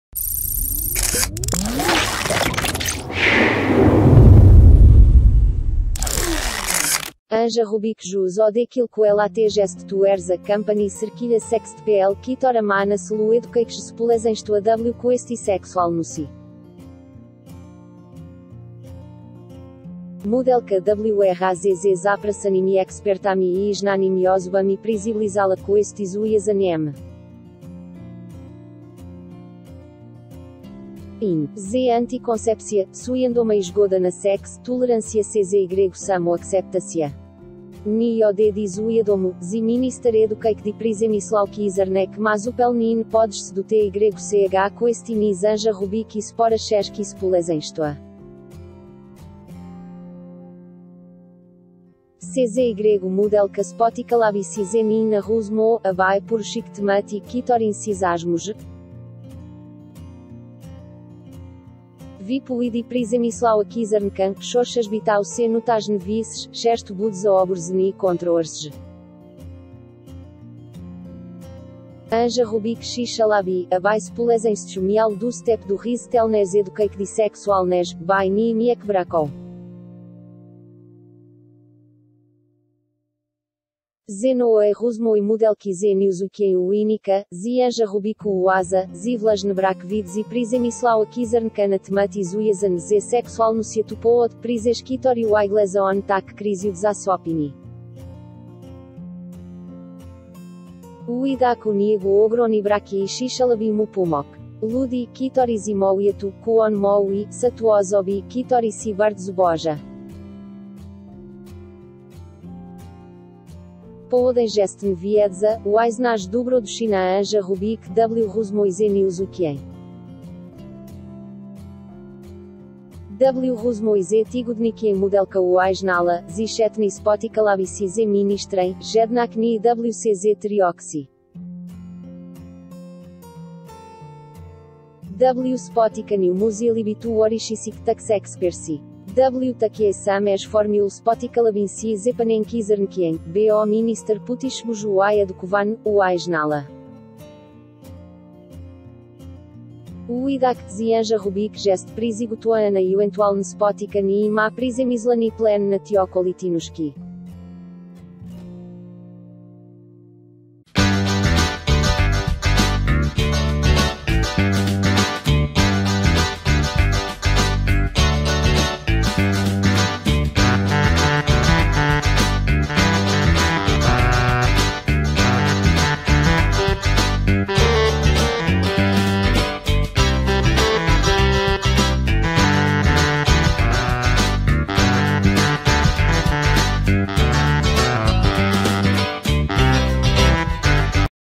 Anja Rubik jest otwarta na spotkanie z ministrem Przemysławem Czarnkiem. Jest jeden warunek. "Chciał
Anja Rubik już od kilku lat jest twarzą kampanii sexedpl, która ma na celu edukację społeczeństwa w kwestii seksualności. Modelka wraz z zapraszanymi ekspertami i znanymi osobami przybliżała kwestie związane m.in. z antykoncepcja, świadomą zgodą na seks, tolerancją czy samoakceptacją. Nie od dziś wiadomo, że minister edukacji Przemysław Czarnek ma zupełnie inne podejście do tych kwestii niż Anja Rubik i spora część społeczeństwa. Czy modelka spotkałaby się z nim na rozmowę, aby poruszyć tematy, którymi się zajmuje?
Wypowiedzi Przemysława Czarnka, chociażby ta o cnotach niewieścich, często budzą oburzenie i kontrowersje. Anja Rubik chciałaby, aby społeczeństwo miało dostęp do rzetelnej edukacji seksualnej, by nie mieć braków. Z nowej rozmowy modelki z "Newsweekiem" wynika, że Anja Rubik uważa, że właśnie brak wiedzy Przemysława Czarnka na tematy związane z seksualnością to powód, przez który wygłasza on tak krzywdzące opinie.

Widać u niego ogromne braki i chciałabym mu pomóc. Ludzie, którzy mówią to, co on mówi, są to osoby, które się bardzo boją. Powodem jest niewiedza - wyznaje dobroduszna Anja Rubik w rozmowie z "Newsweekiem".
W rozmowie z tygodnikiem modelka wyznała, że chętnie spotkałaby się z ministrem, jednak nie w cztery oczy. W spotkaniu musieliby towarzyszyć także eksperci.

W takiej samej formule spotkałabym się z panem Czarnkiem, bo minister potrzebuje wyedukowania - wyznała.
Widać, że Anja Rubik jest przygotowana na ewentualne spotkanie i ma przemyślany plan na tę okoliczność.  Anja Rubik jest otwarta na spotkanie z ministrem Przemysławem Czarnkiem. Jest jeden warunek. "Chciał | Aktualności 360 | Poland News